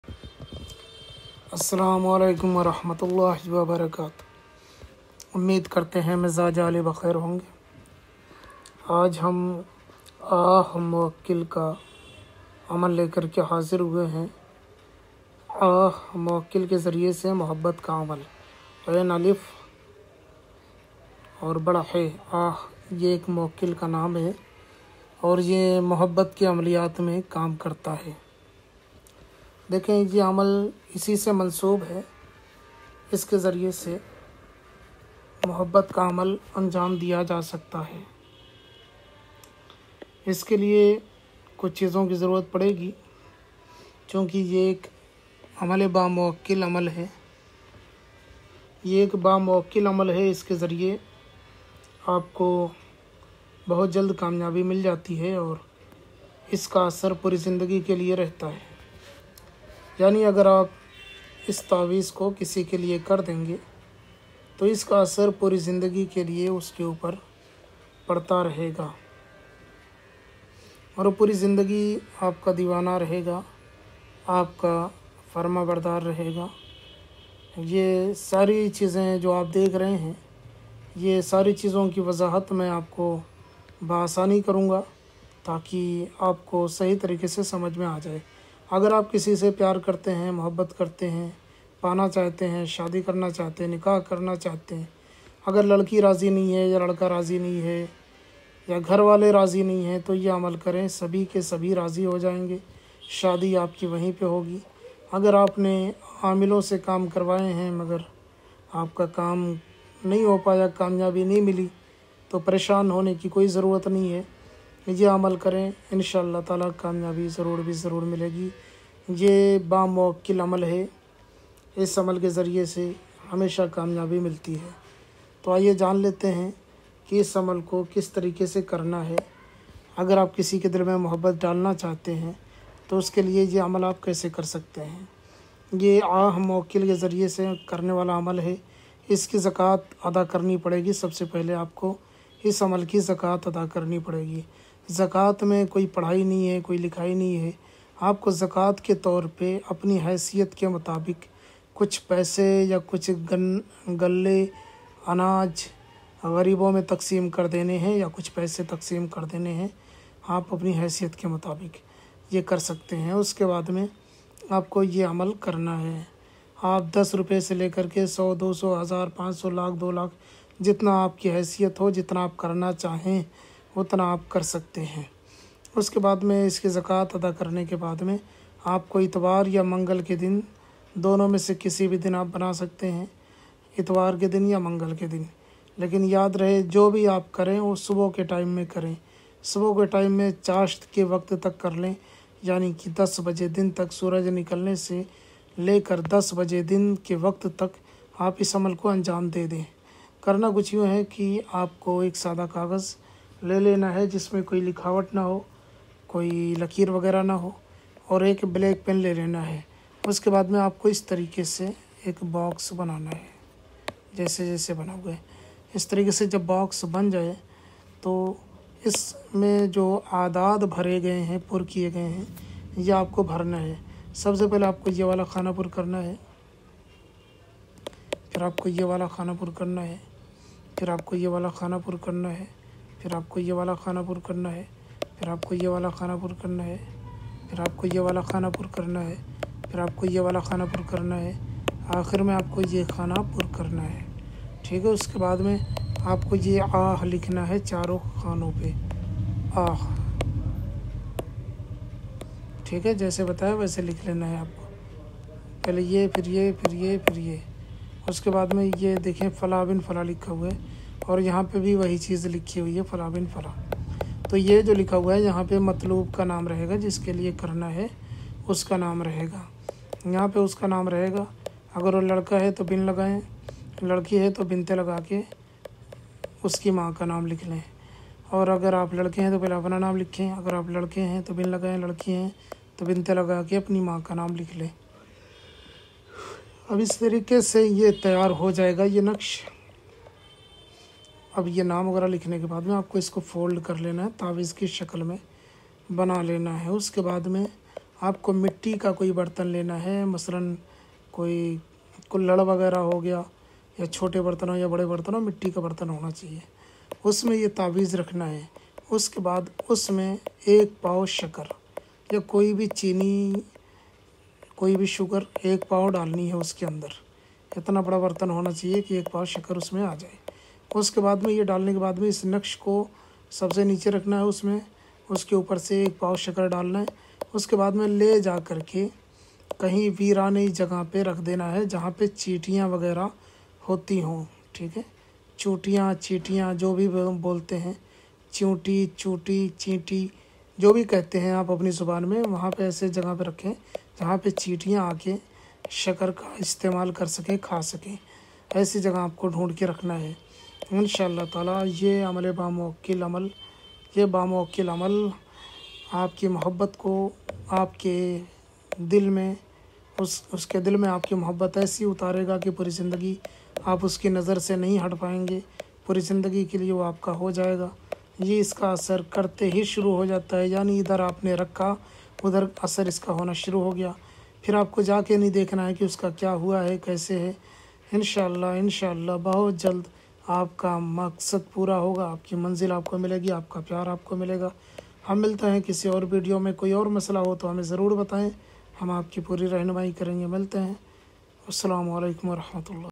Aah mowkkil ke zariye mohabbat ka amal || ye amal kr lo aap ki mohabbat aap ko mil jaye gi
About this video: ye video Mohbbat ka video h is me hm ne bataya h ki aah mowkkil ke zariye mohabbat ka amal kaise karen ye amal bahut aasan h is me hm ne bataya h ki ap ki mohabbat aap ko kaise mil sakti h is amal ko karen kamiyabi zarur mile gi ||